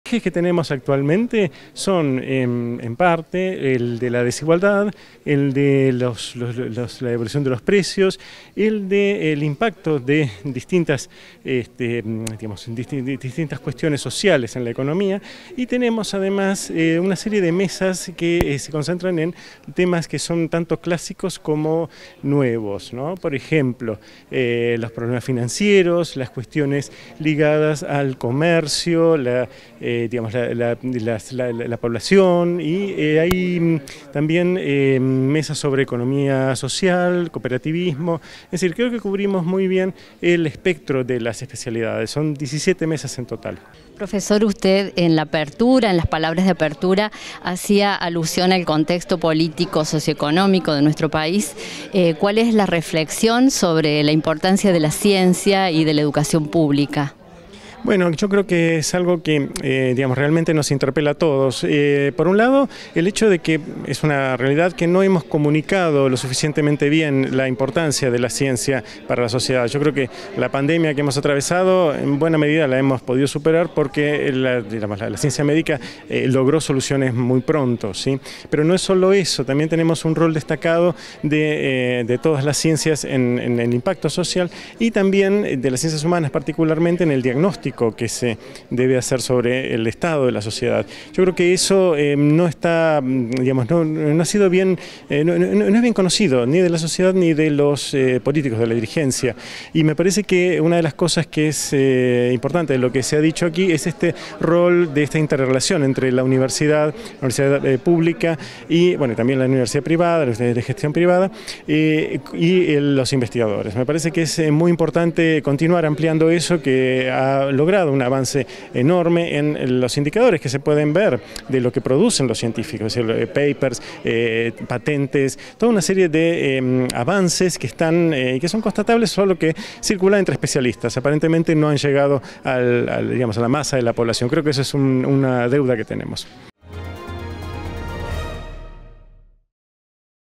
LAS DESIGUALDADES ES UNO DE LOS EJES DE DEBATE EN LAS JORNADAS DE HISTORIA ECONÓMICA
JULIO DJENDEREDJIAM- PRESIDENTE ASOCIACIÓN ARGENTINA DE HISTORIA ECONÓMICA
UniRio TV
Universidad Nacional de Río Cuarto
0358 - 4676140